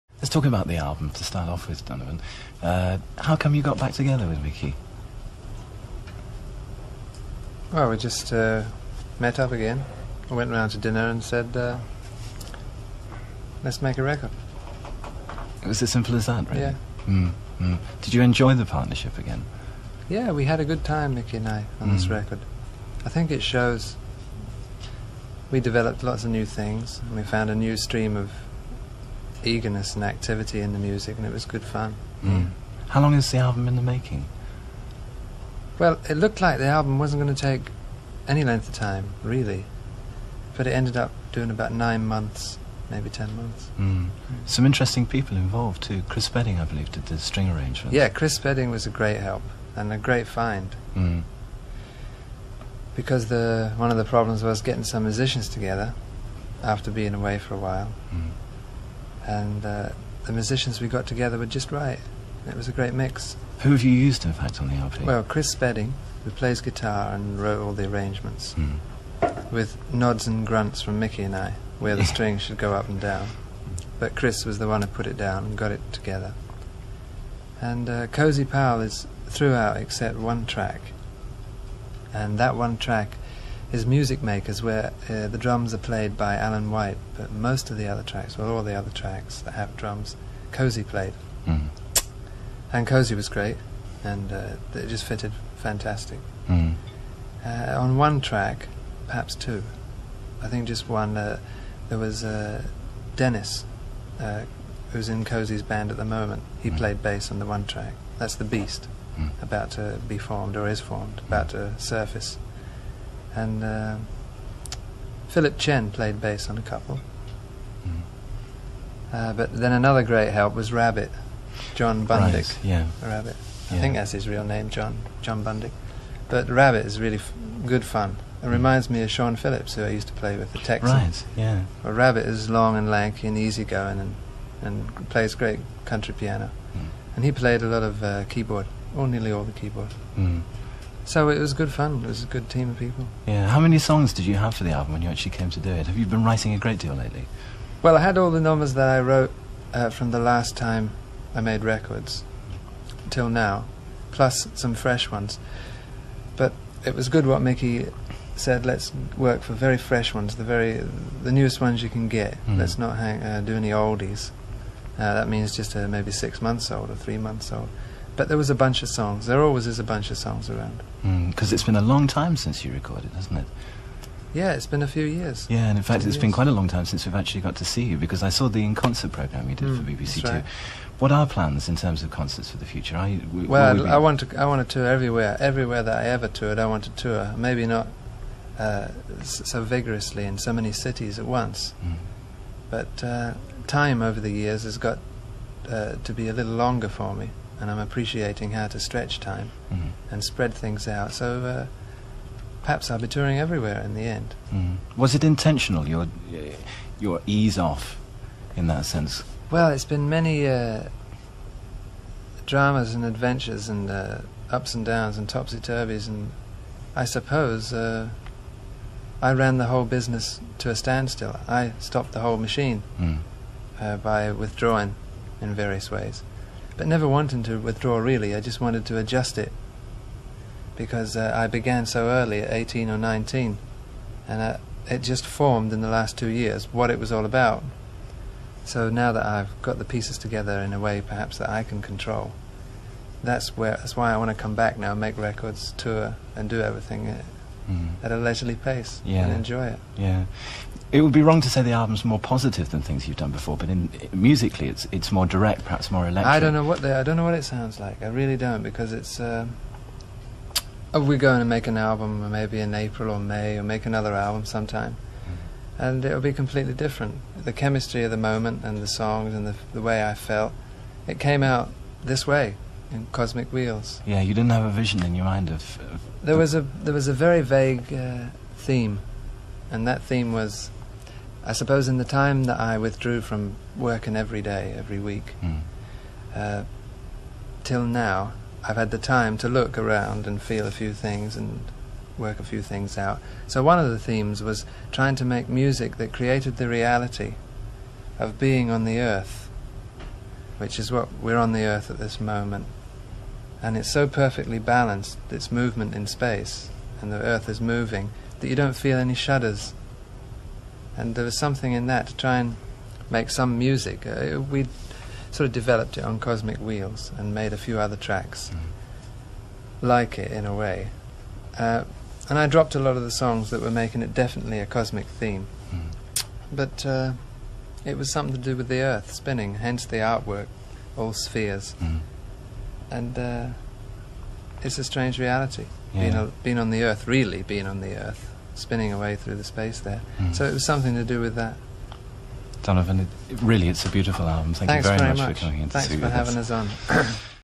Donovan - rare interview (1973)
Donovan in 1973 talking with Whispering Bob Harris about his reuniting with original producer Mickey Most for what would be his last charting album. Emerging from the British folk scene in 1965, despite unfair Dylan comparisons, Donovan proved otherwise in the 1960's with a series of brilliant chart-topping singles that rank among the finest to come out of the British 1960's pop era.